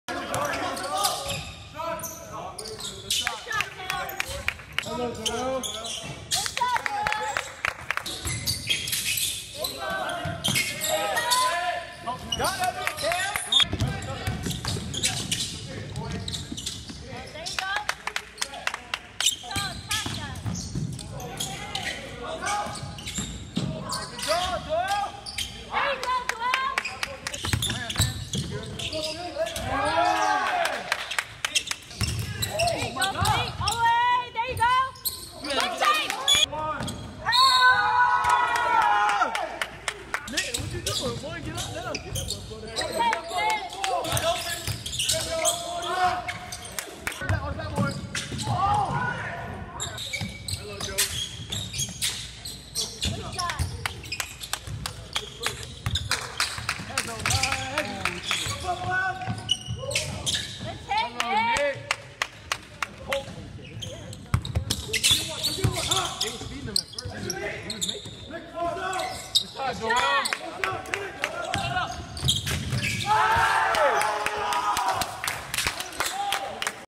PHS Academy RED vs IMPACT ACADEMY PG
The knights Take down Impact Academy on the road for a double digit victory.